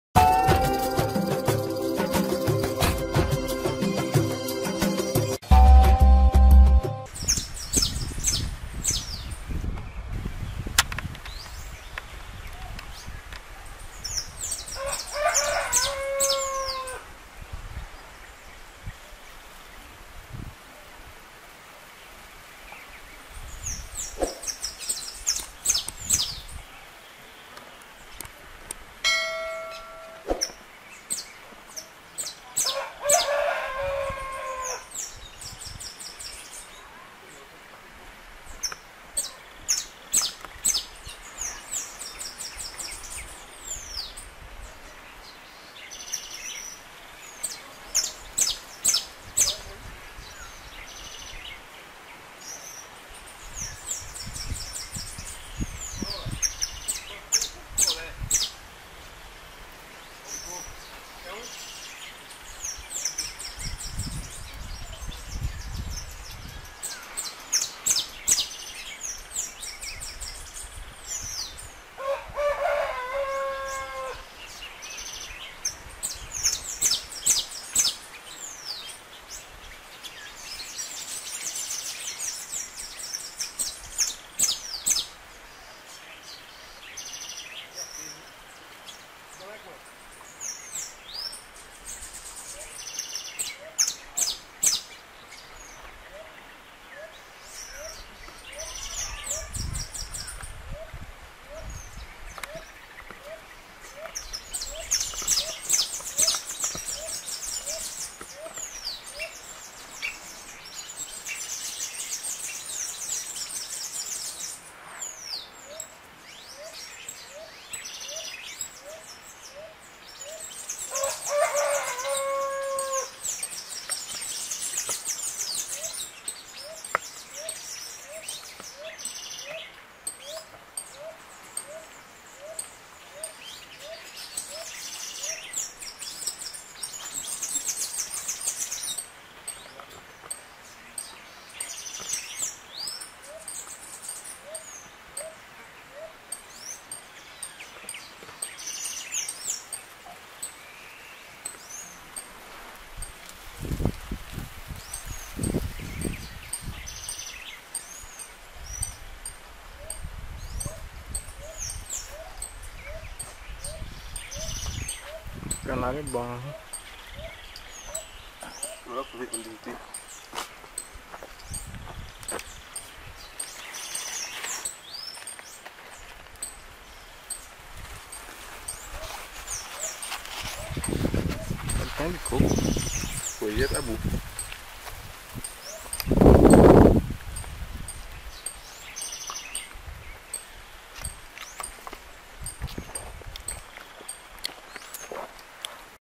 CANARIO DA TERRA CANTA E ENCANTA canto estalo (Sicalis flaveola)
O canário-da-terra-verdadeiro (Sicalis flaveola), também é conhecido como canário-da-horta, canário-da-telha (Santa Catarina), canário-do-campo, chapinha (Minas Gerais), canário-do-chão (Bahia), canário-do-reino (Ceará), coroinha e cabeça-de-fogo, é uma ave admirada pelo canto forte e estalado e por isso é frequentemente aprisionado como ave de cativeiro (está entre as 10 mais apreendidas, segundo o IBAMA), mesmo tal ato sendo considerado crime federal pela Lei de Crimes Ambientais (Lei 9.605/98). Graças à ação das autoridades e da conscientização da população, registros do canário-da-terra-verdadeiro vêm se tornando mais frequentes nos últimos anos.